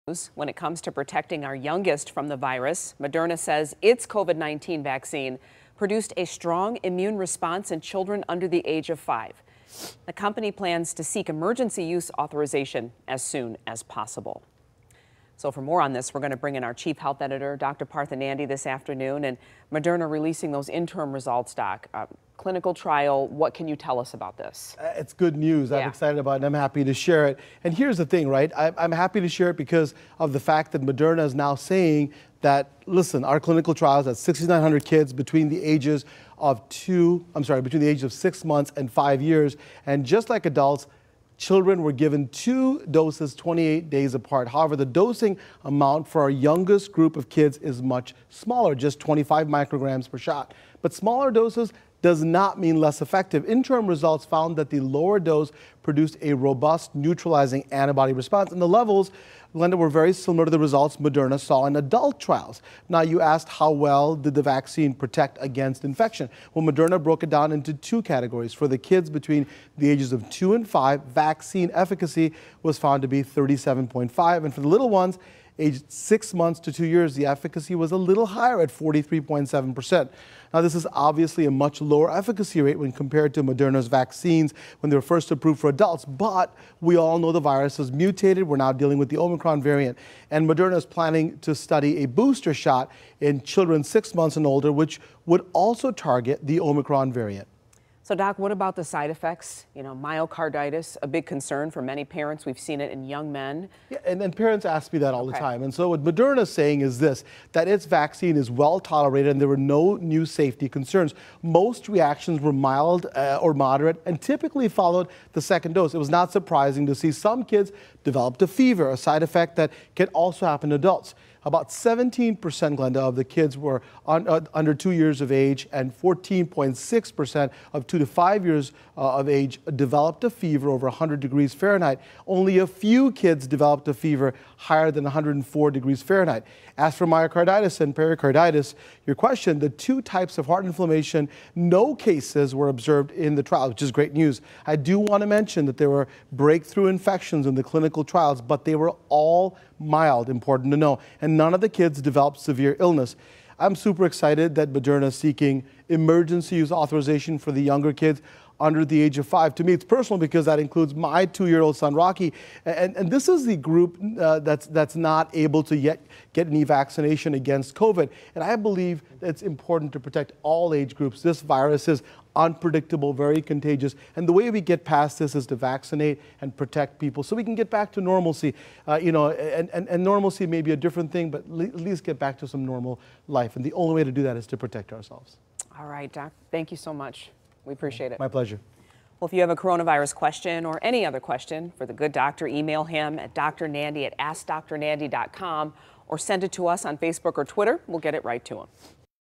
Moderna says COVID-19 vaccine performed well in trial of young children
Moderna says its COVID-19 vaccine produced a strong immune response in children under the age of 5. The company plans to seek emergency use authorization as soon as possible.